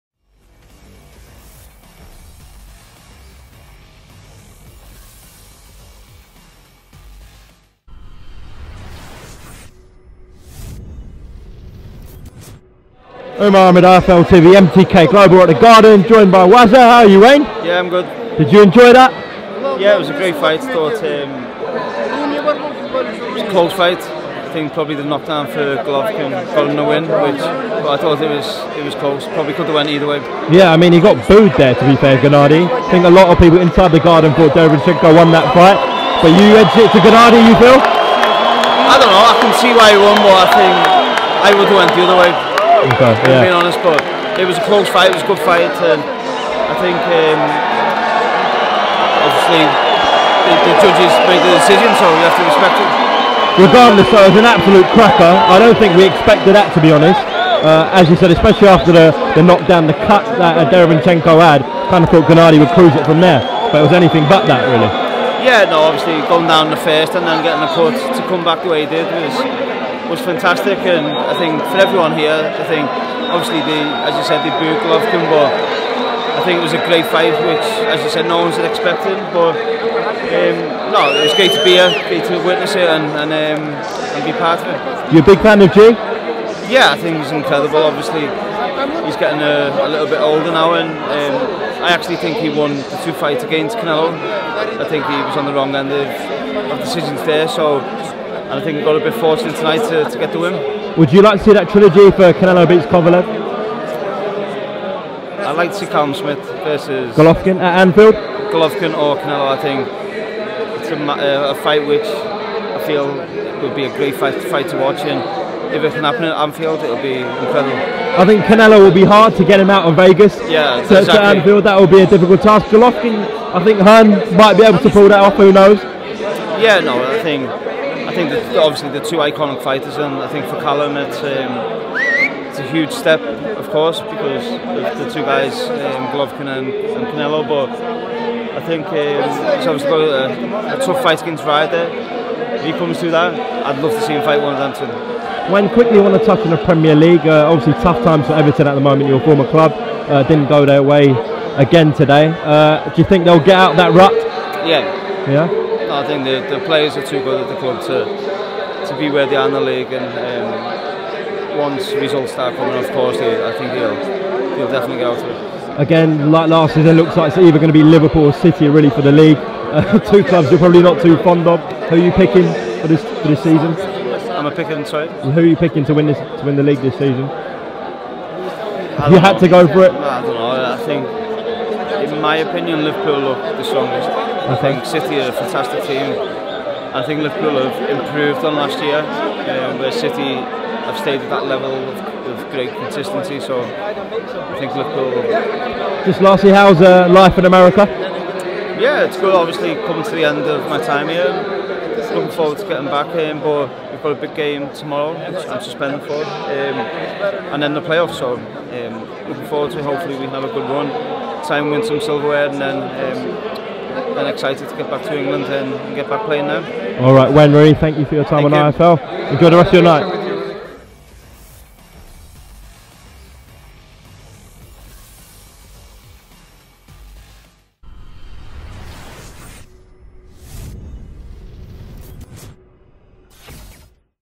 'I WANT TO SEE GGG v SMITH AT ANFIELD!' - WAYNE ROONEY REACTS TO GOLOVKIN'S UD WIN & TALKS EVERTON
*INTERVIEW BY UMAR AHMED @ MADISON SQUARE GARDEN*